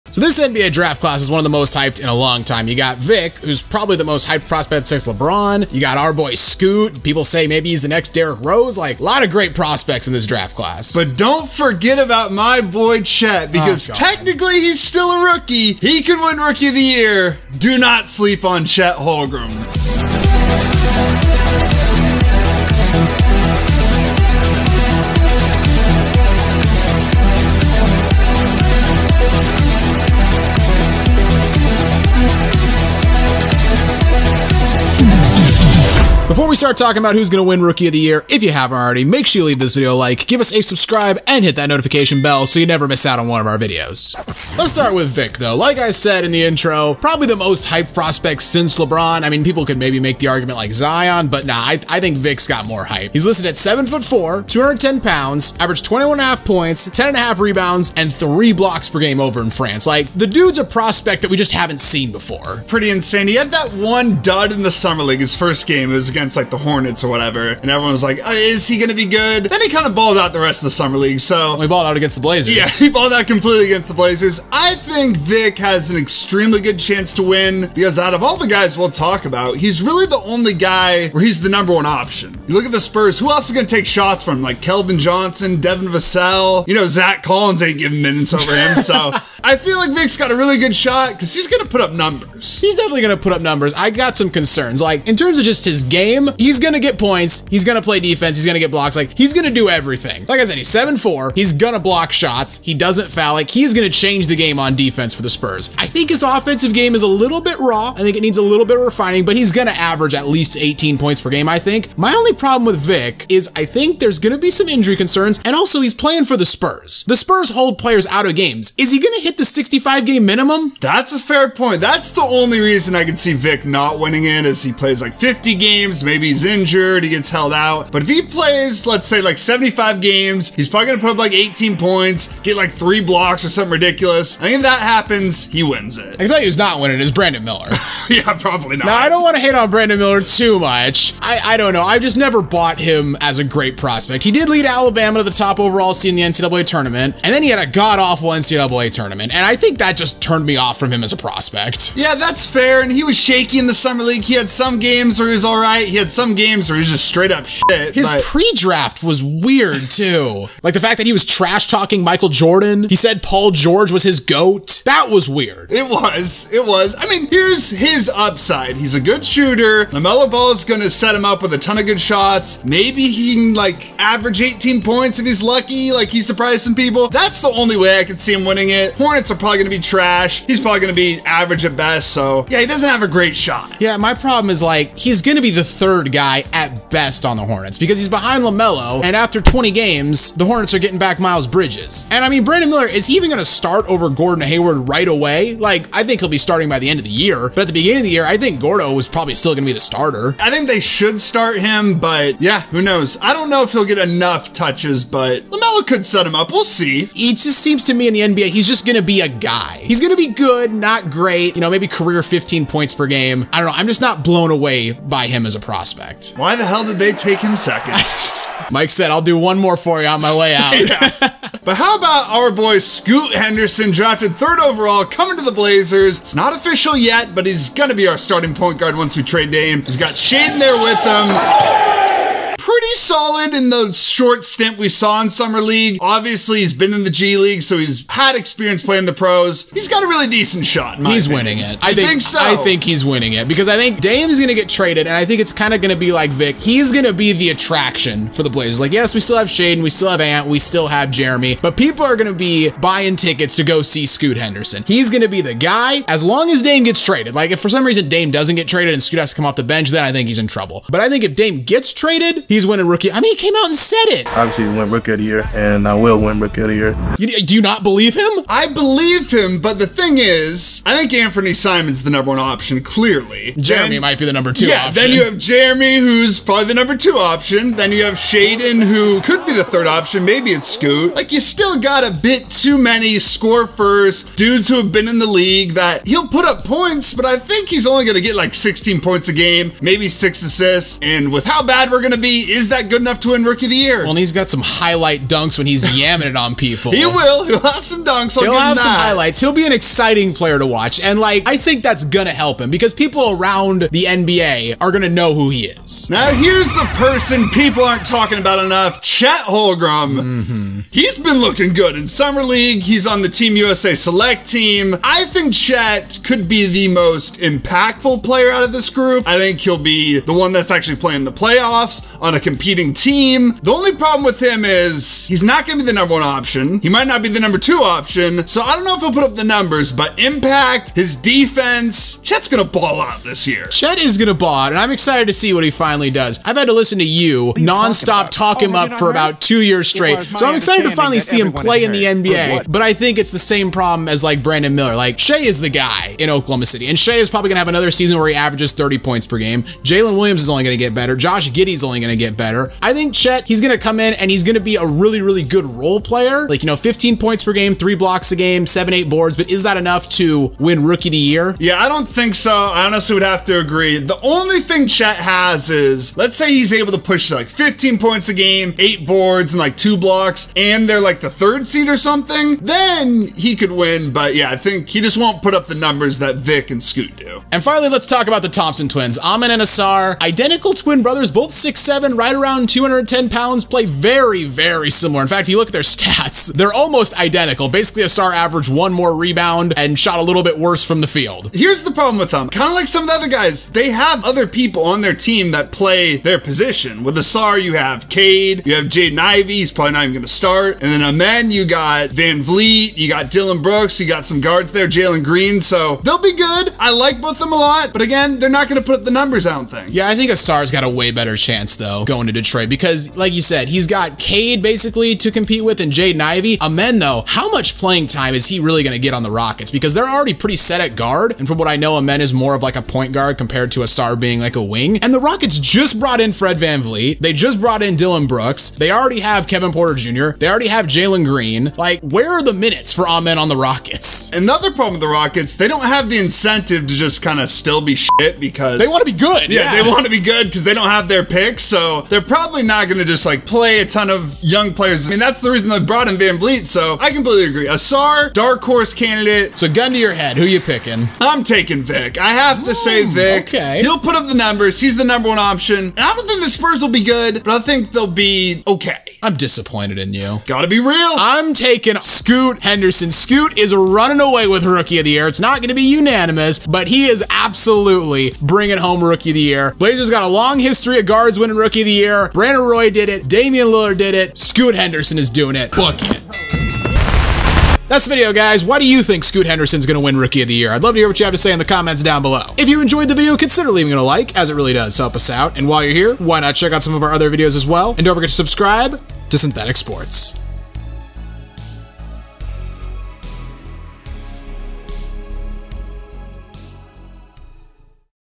Who’s Winning Rookie of the Year?
The 2023 NBA Rookie class is one of the most intriguing in recent memory. Led by Victor Wembenyama who is arguably the most hyped prospect since LeBron. Wembenyama is a 7'4 center with versatility and agility so rare for a player his size. He is joined in this class by Brandon Miller, out of Alabama, and Scoot Henderson, from the G-League Ignite, both of whom have superstar potential. And that's before we start talking about Chet Holmgren or the Thompson twins. So who wins this seasons Rookie of the Year?